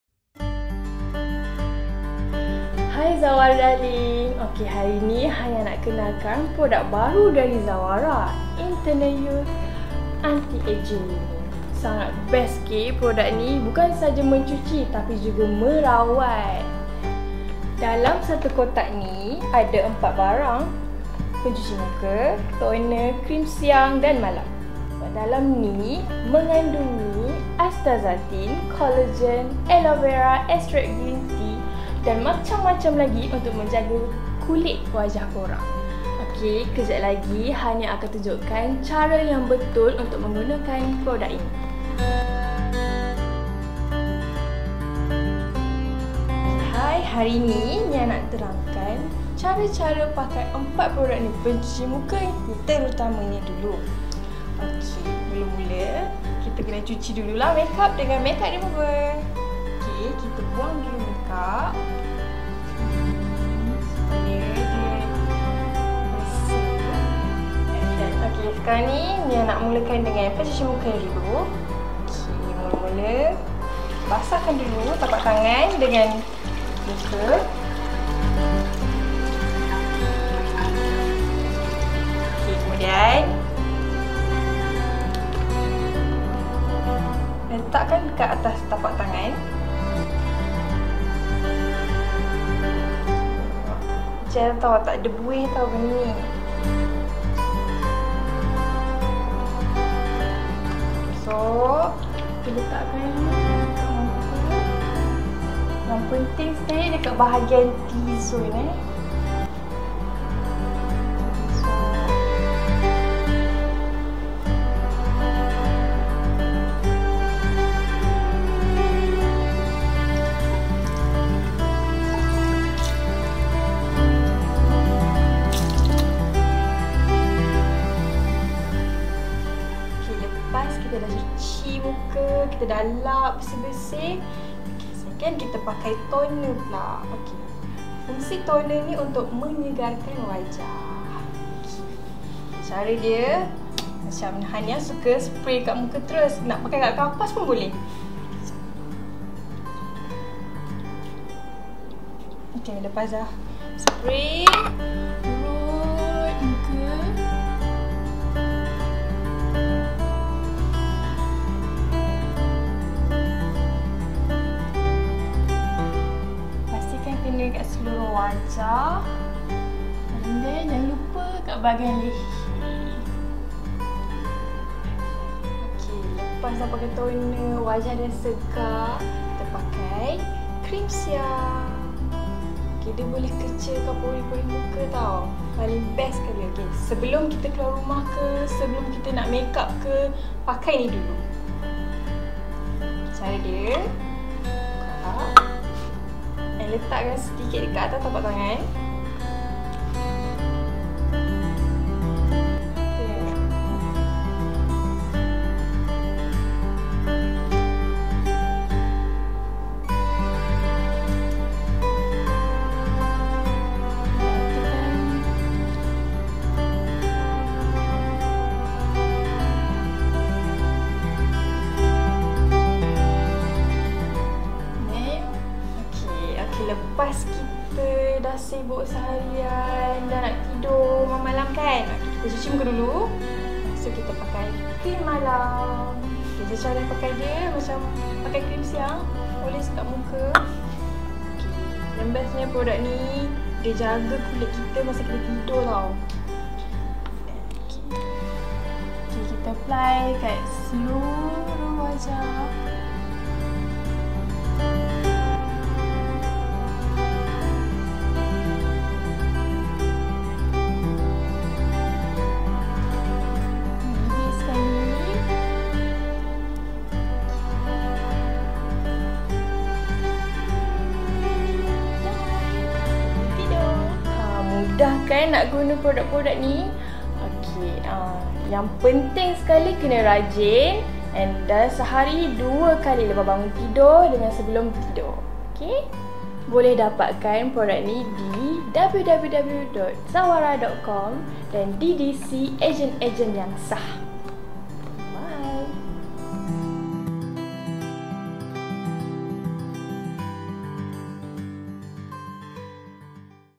ZAWARA ETERNAL YOUTH ANTI-AGING KIT
ZAWARA Eternal Youth adalah untuk Anti-Penuaan dalam meningkatkan jangka hayat sel-sel kulit, kulit akan mempunyai penampilan yang lebih muda dan berseri. Gabungan bahan aktif mempunyai astaxanthin, aloe vera, minyak pokok teh, minyak biji jojoba dan ekstrak sel stem epal untuk kelihatan lebih segar, menegangkan kulit, meningkatkan keanjalan, dan melicinkan garis-garis halus dan kedutan yang bersinar muda.

Astaxanthin ni adalah King of antioksidan terbaik di dunia!
☀ 6000 X lebih Hebat Dari Vitamin C
☀ 1000 X lebih kuat dari Vitamin E
☀ 40 X lebih kuat dari Beta Karoten
☀ 17 X lebih kuat dari Grape Seed
Sebab itu kami jadikan Astaxanthin ini sebagai kandungan khas di dalam produk skincare ZAWARA iaitu ZAWARA Eternal Youth Kit ini
Untuk penjagaan kulit yang sihat, cantik dan kekal muda, jom dapatkan produk skincare ZAWARA sekarang!
Dapatkan Hanya Dari Agen-agen Sah ZAWARA® Yang Sangat Terlatih & Dipercayai.